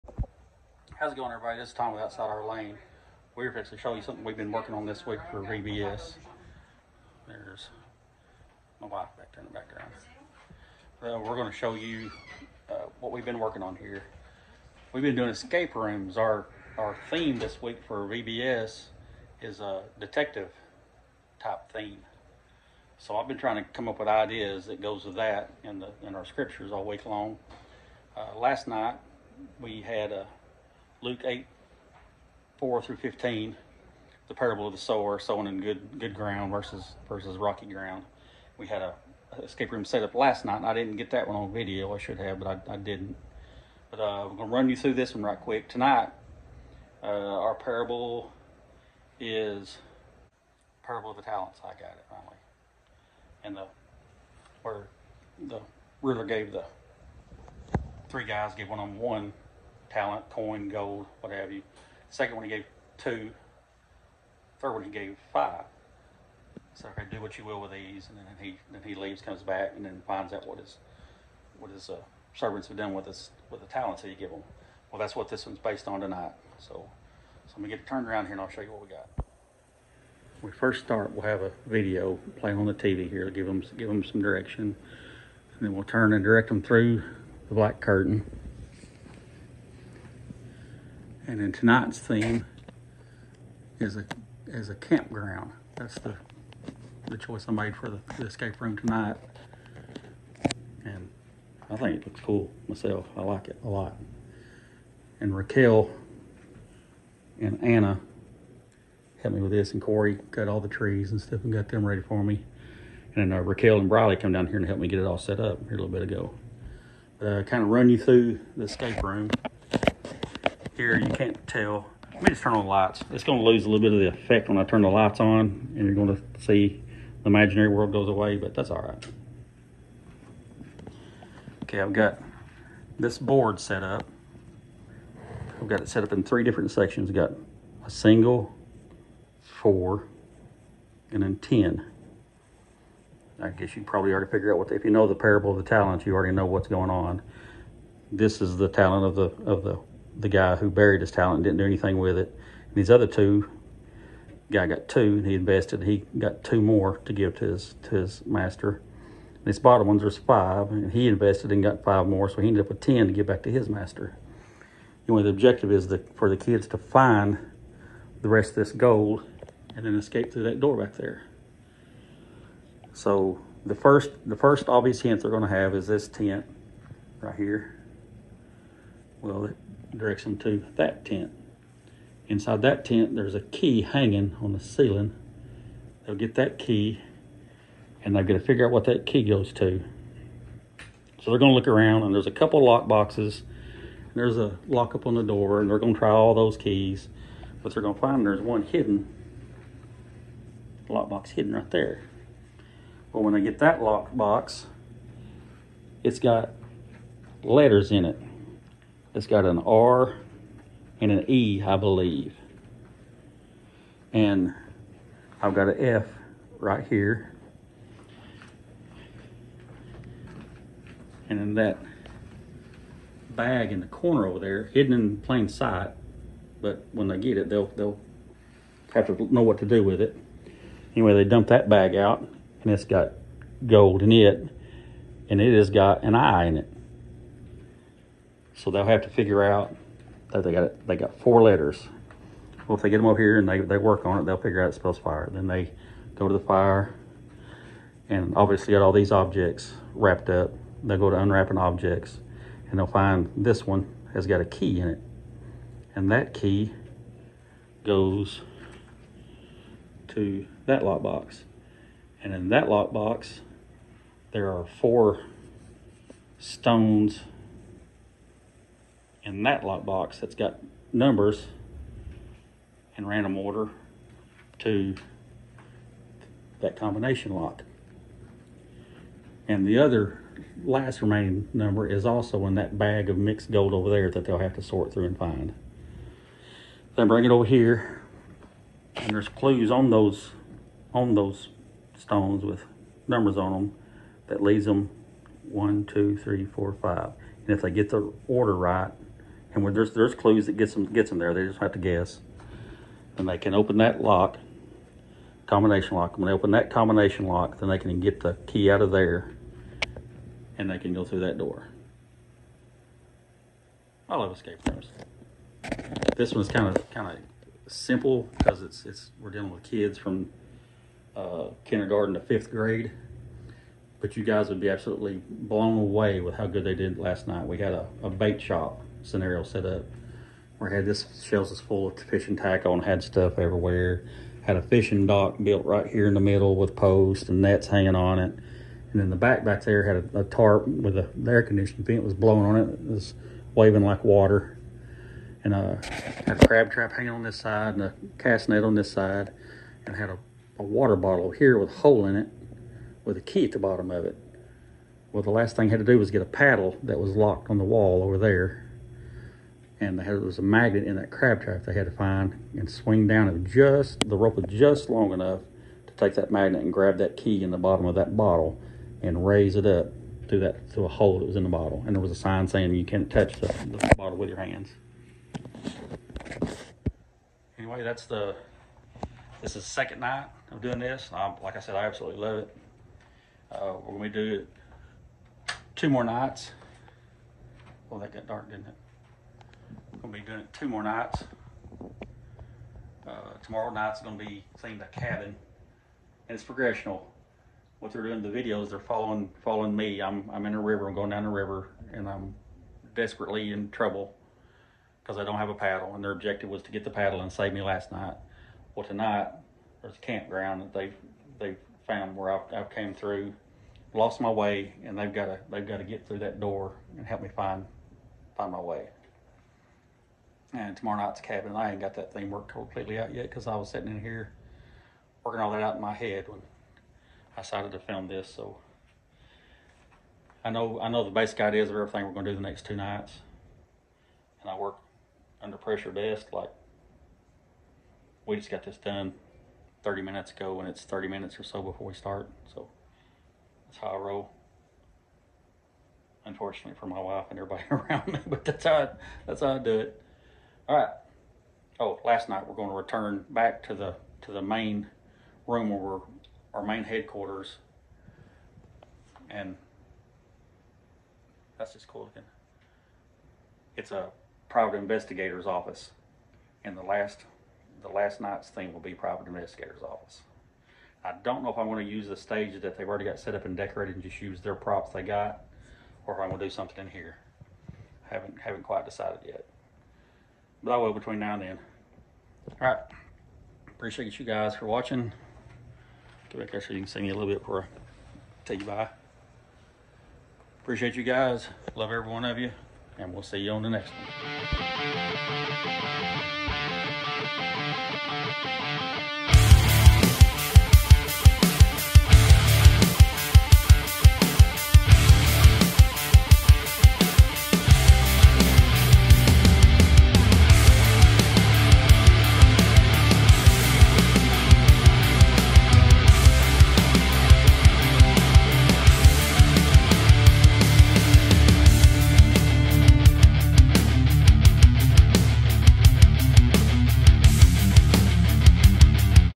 How to make an escape room out of simple things!
We decided to create our own diy escaperoom for our vacationbibleschool this week. This video will explain the room and what you need to do one for your own kids group.